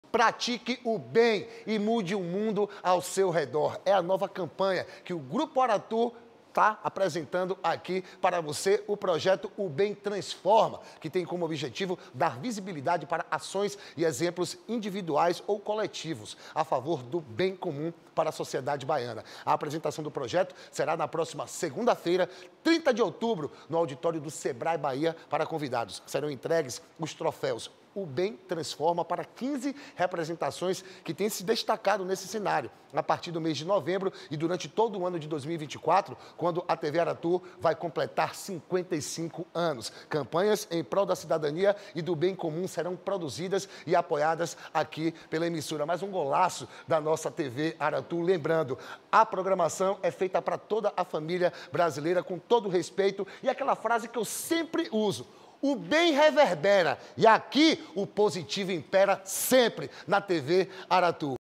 O Bem Transforma: Grupo Aratu lança nova campanha a favor do bem social
“Pratique o bem e mude o mundo ao seu redor”. Esse será o mote da nova campanha que o Grupo Aratu promove para apresentar o projeto O Bem Transforma, que tem como objetivo principal dar visibilidade estruturada para ações e exemplos individuais ou coletivos a favor do bem comum para a sociedade baiana.

A apresentação do projeto será na próxima segunda-feira, 30 de outubro, no auditório do Sebrae Bahia para convidados.

Para estimular a continuidade dessas iniciativas e motivar que mais pessoas se inspirem nessas referências do “empreendedorismo pelo bem”, a Aratu fará algumas homenagens e entregará o troféu “O Bem Transforma” para 15 representações que têm se destacado nesse cenário de ações positivas e humanas.

“Vamos começar nossa campanha da melhor forma possível, agradecendo a pessoas e organizações que têm se dedicado a praticar o bem e mudar o mundo de inúmeras pessoas com seus exemplos e atitudes. Acreditamos que essas ações devem reverberar e alcançar o máximo de pessoas“, pontua João Gomes, superintendente do Grupo Aratu.